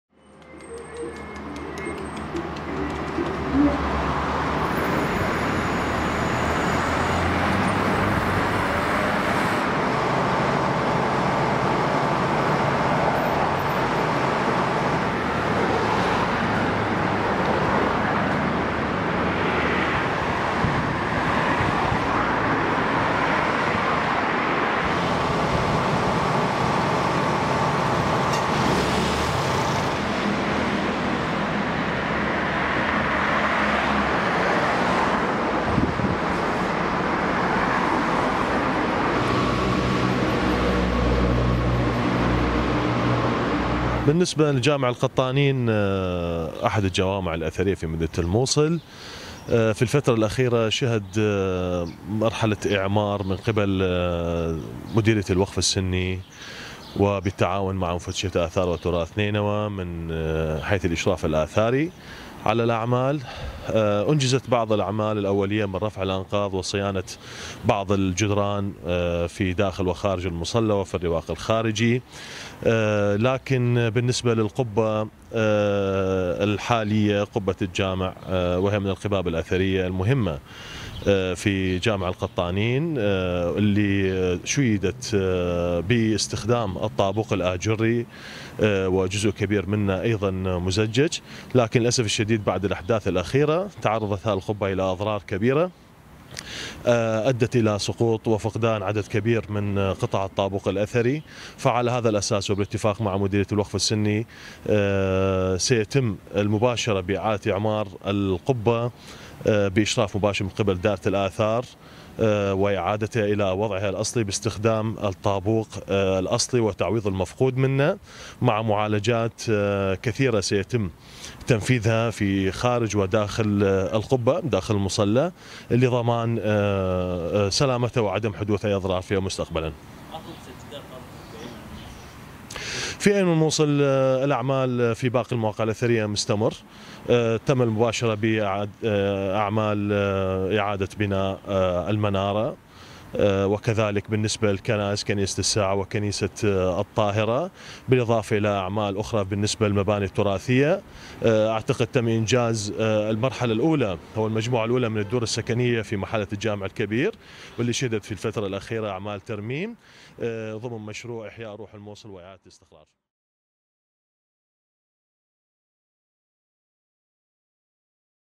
مفتشية آثار نينوى: جامع القطانين بالموصل القديمة شهد أعمال اعمار وقريبا سنباشر بإعادة اعمار القبة
مفتشية آثار نينوى: جامع القطانين بالموصل القديمة شهد أعمال اعمار أولية.. وقريبا سنباشر بإعادة اعمار القبة